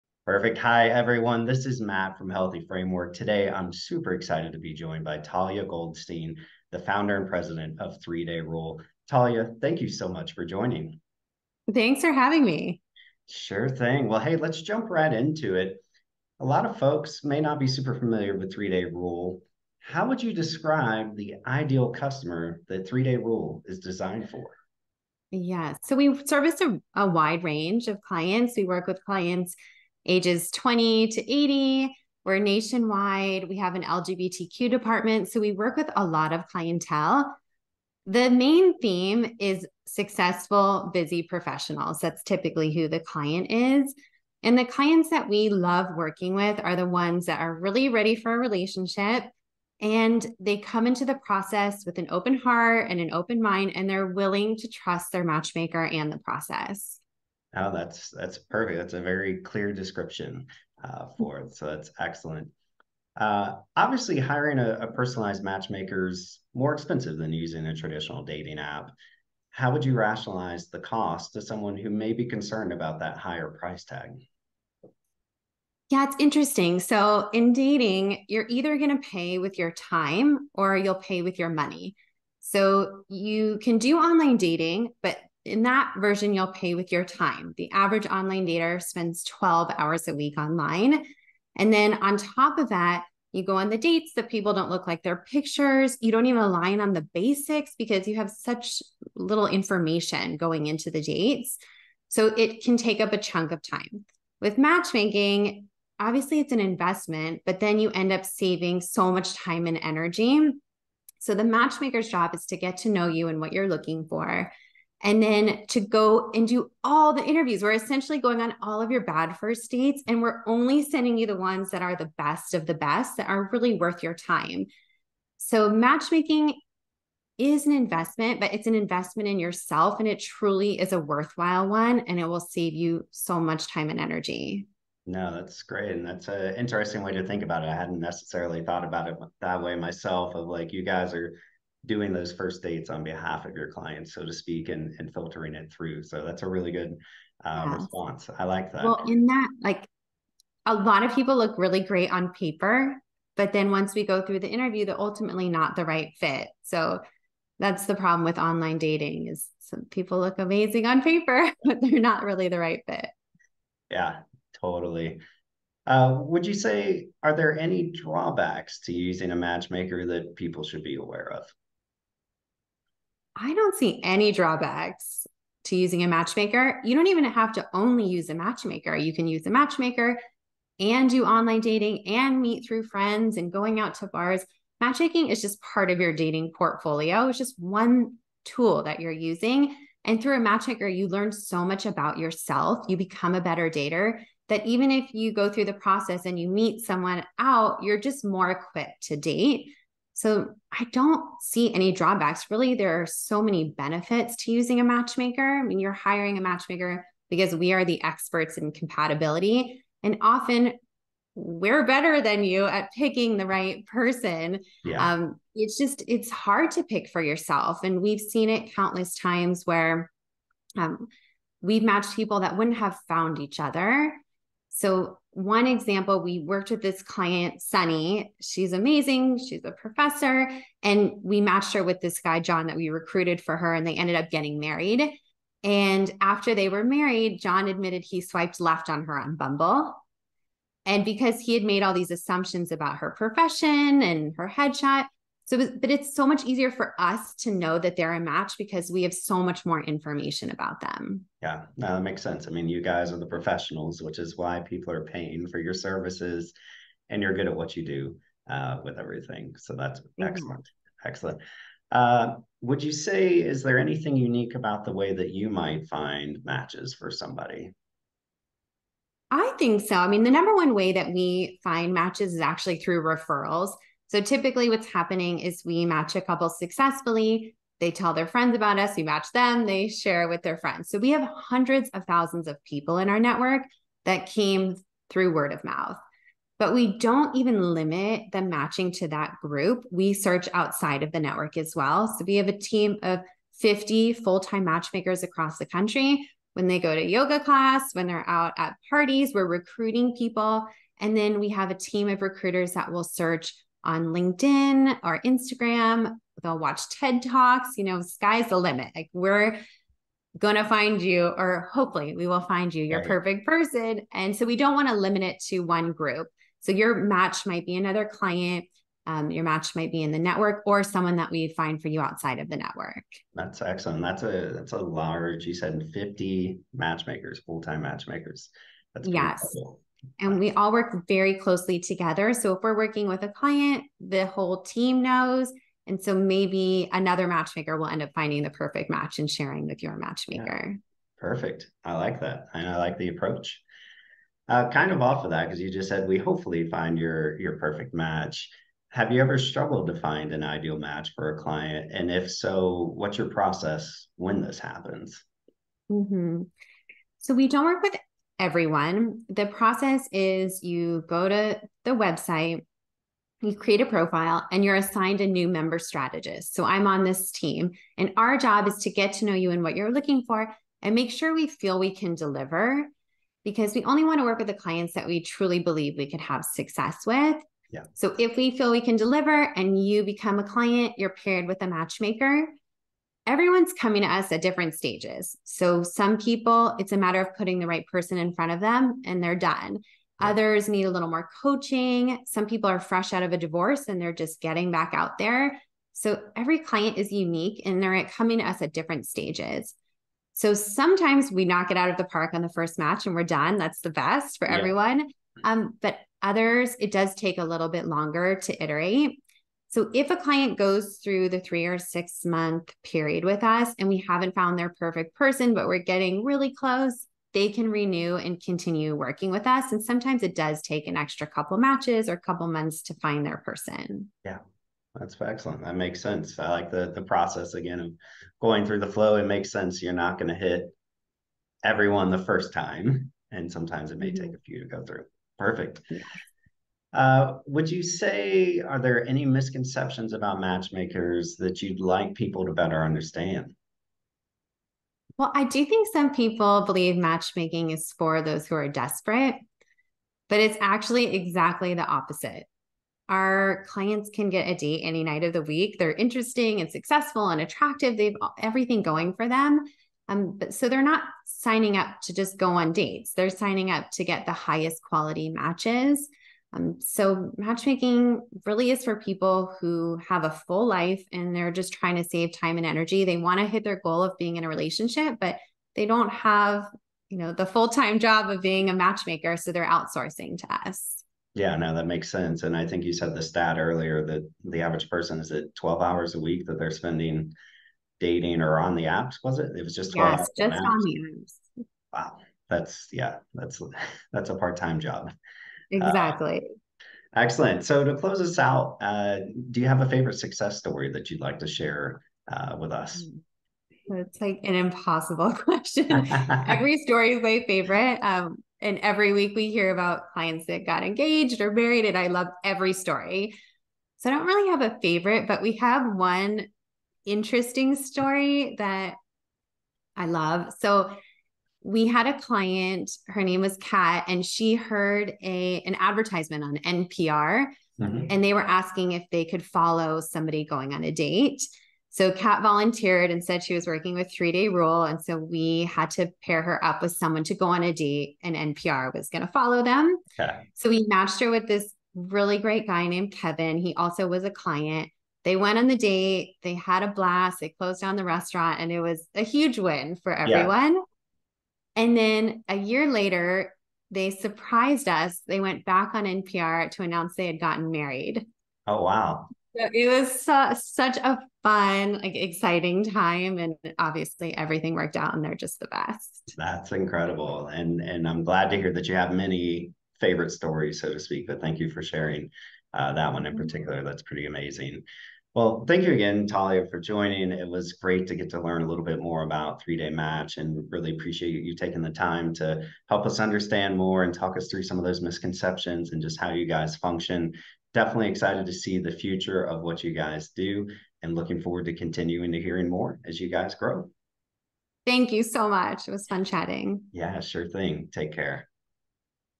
Interview with Three Day Rule Matchmakers Founder and President Talia Goldstein
Click Here to Schedule a Free Call With Three Day Rule ➡️

Our VP of Partnerships Matthew J. Seymour sits down with matchmaking company Three Day Rule's Founder and President Talia Goldstein. The two discuss the ideal Three Day Rule customer, the matchmaking industry as a whole, and the pros and cons of using a service like Three Day Rule (3DR) versus its competitors.